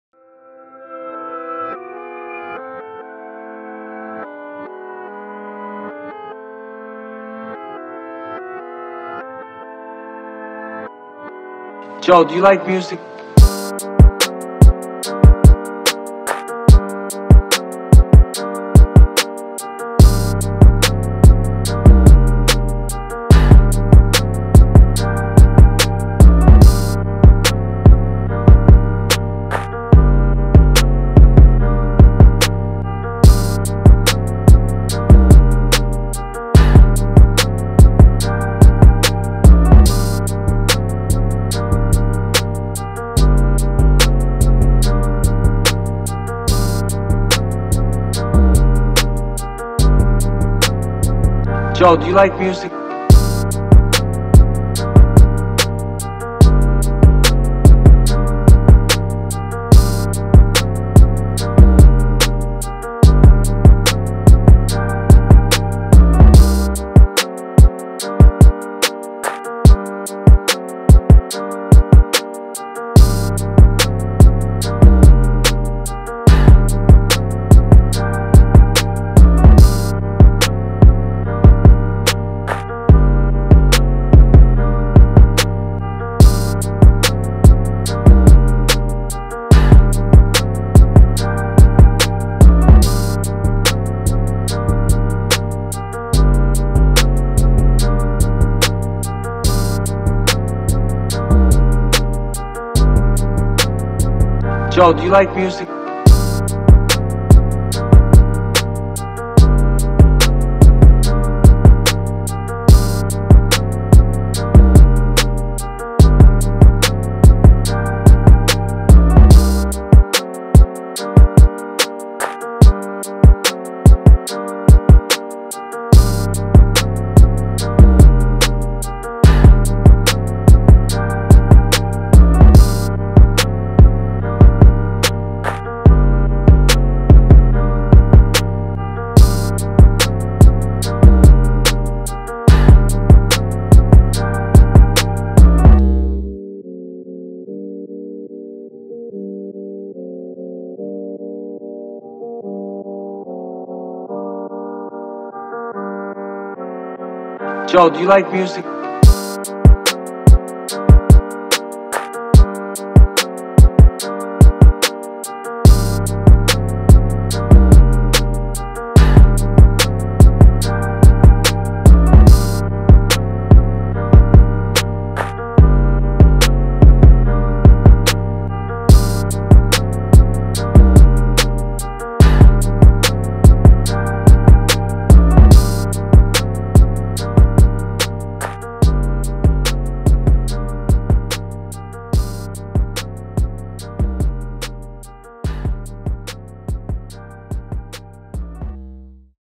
[FREE] J. Cole/Kendrick Lamar Type Beat | (Boom Bap/Old School Type Beat)
A J.cole/ Kendrick Lamar Type Beat with a Trap Flavour